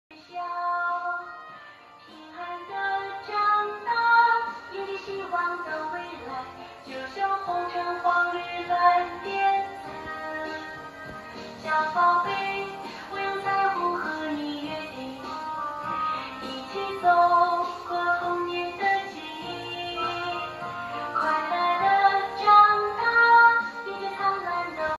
EPARK 6 Seats Aircraft Ride Attraction Kids Mechanical Ride Airplane Rotation Mini Aircraft Rides
"Model NO.:EP-K145
Size:340*340*150 CM
Voltage: 220V
Power: 500W
Material: PP+ABS Weight: 170KG
Player: 6"